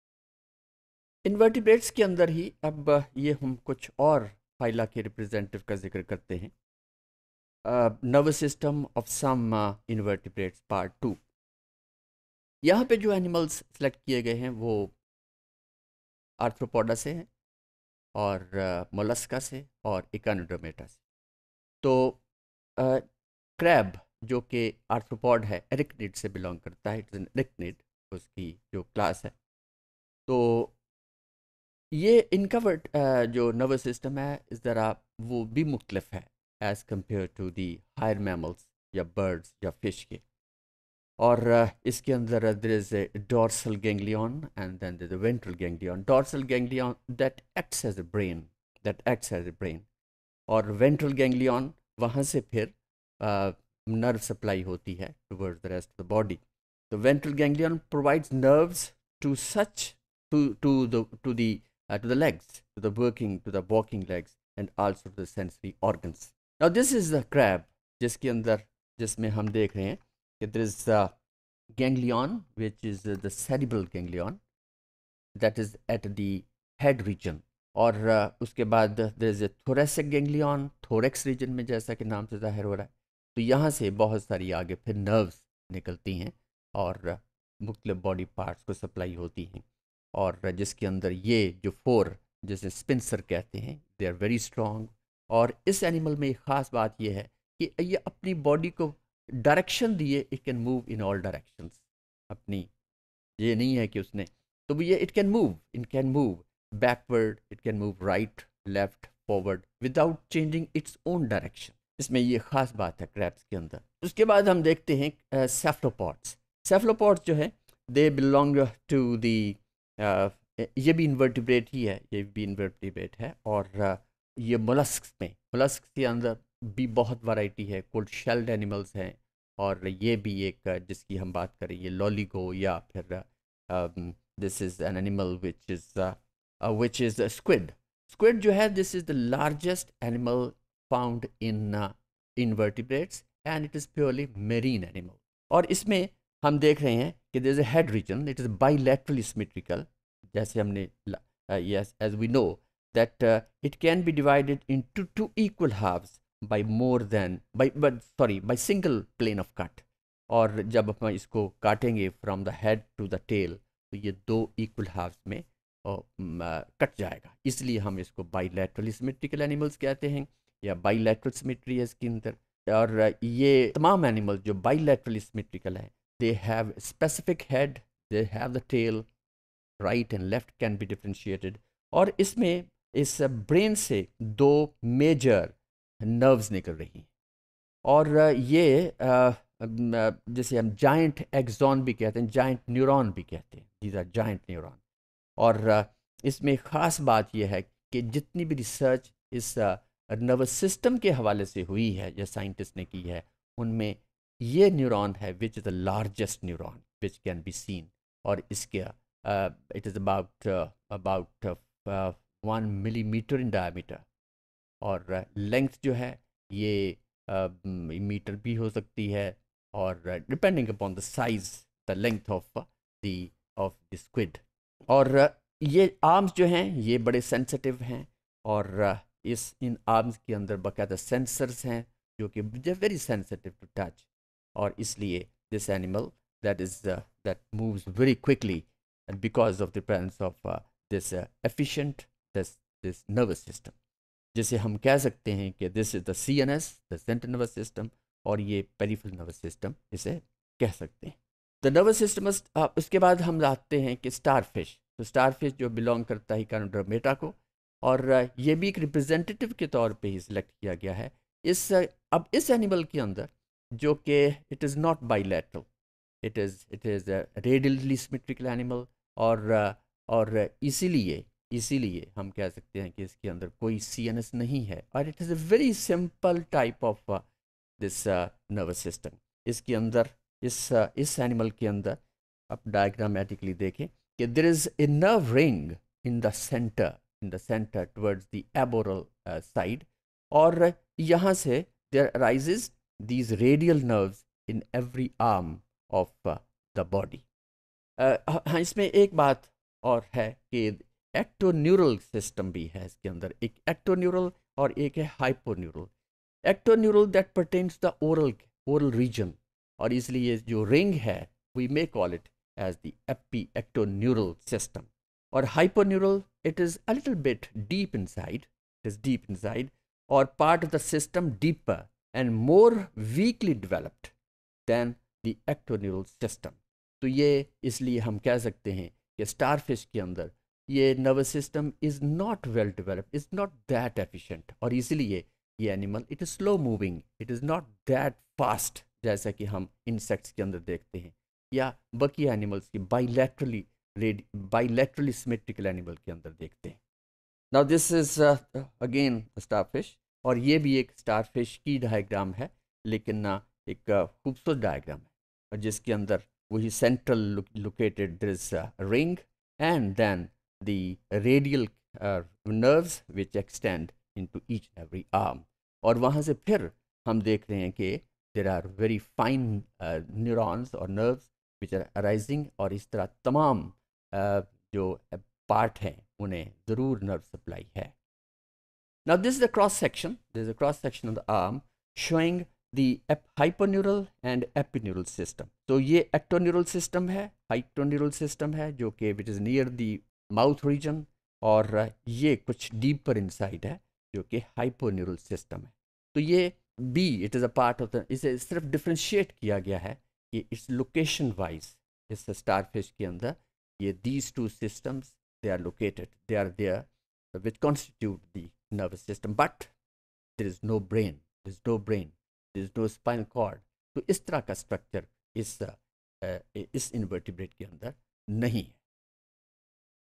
Nervous System of Some Invertebrates 2 | Animal Form & Function 1 (Theory) | ZOO516T_Topic046
Topic046: Nervous System of Some Invertebrates - 2,
By Dr. Javed Iqbal